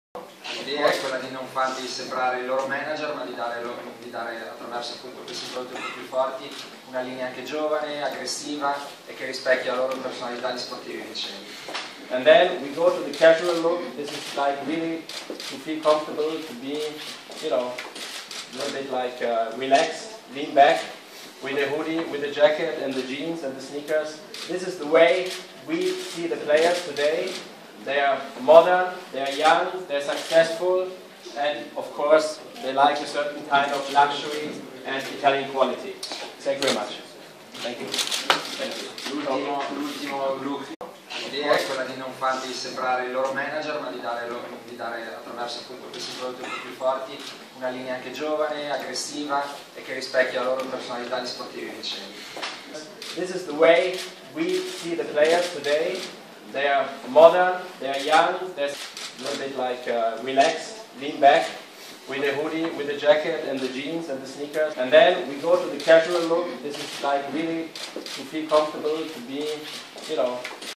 LAROMA24.IT Presentazione divise Philipp Plein, Lamela scherza con Osvaldo 08.11.12
LAROMA24.IT -Presentazione divise Philipp Plein: sfilano Lamela, Osvaldo, Balzaretti e Stekelenburg 08.11.12 .

Nel quartier generale di Trigoria dove la squadra della Roma è solita allenarsi, si è svolta la presentazione delle divise ufficiali sponsorizzate dallo stilista .